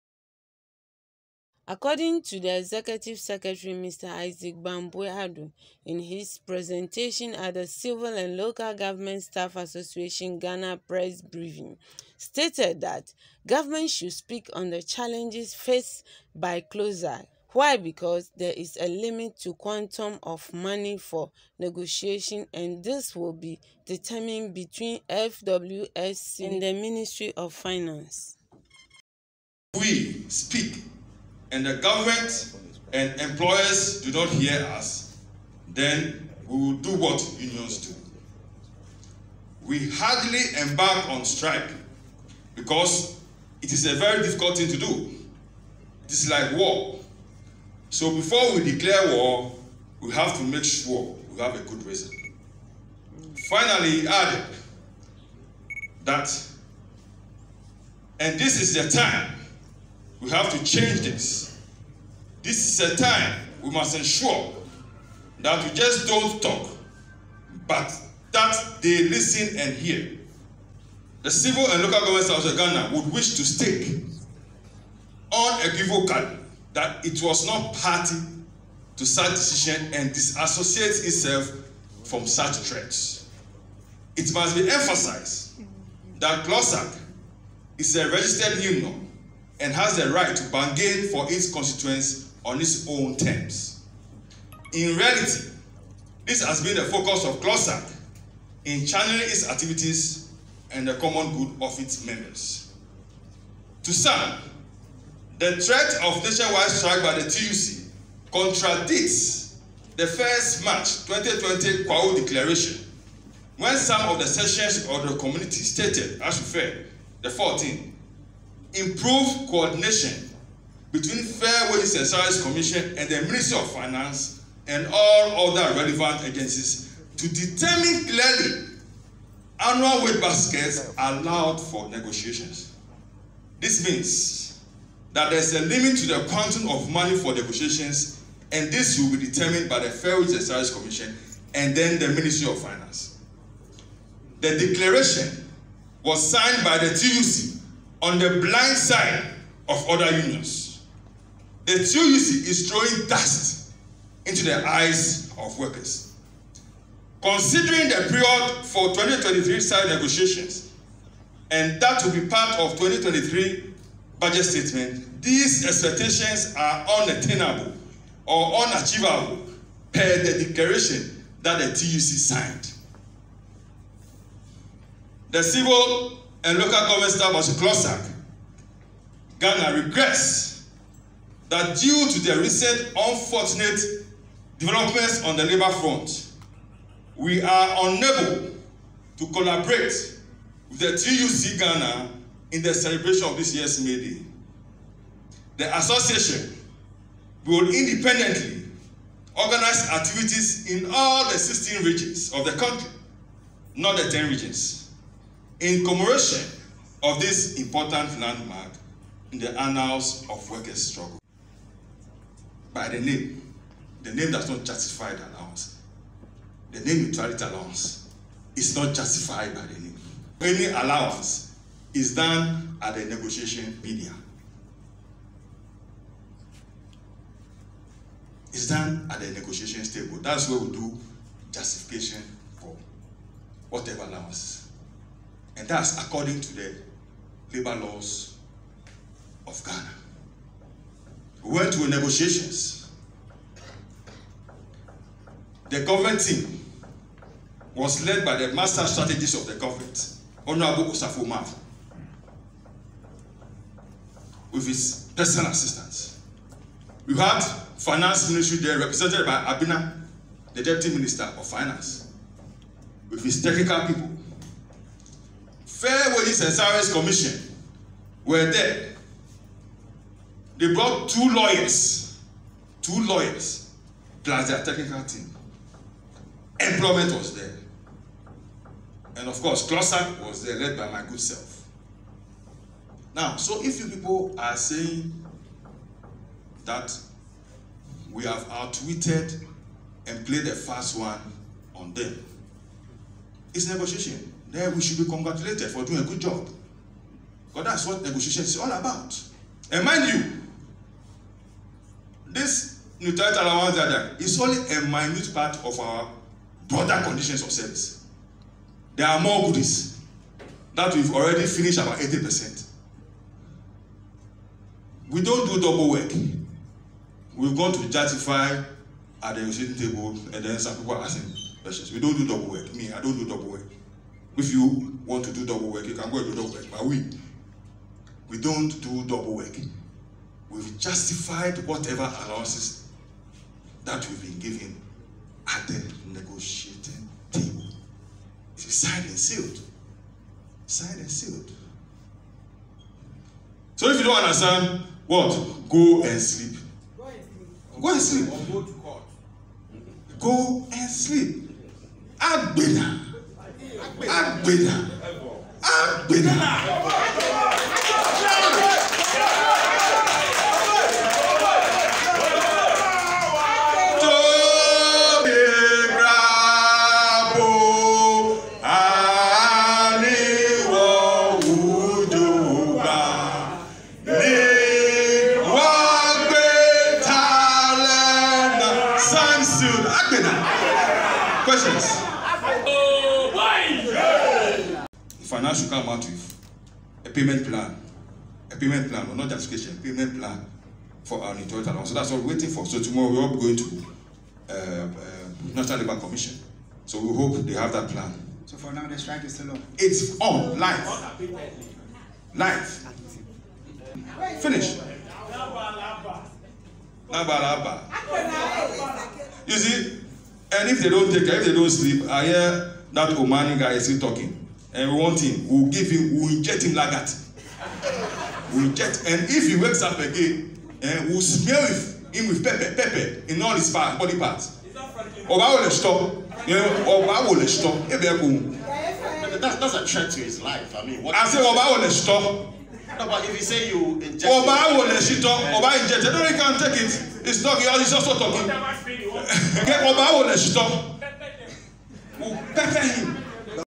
CLOGSAG CALL FINANCE MINISTER TO COME OUT WITH PAYMENT AND NOT JUSTIFICATION
According to the Executive Secretary Mr.Issac Bampoe Addo in his presentation at the Civil and Local Government Staff Association Ghana press briefing, stated that Government should speak on the Challenges faced by clogsag.

Furthermore, established that it's because there's is a limit to quantum of money for negotiation and this will be determined between FWSC and the ministry of finance.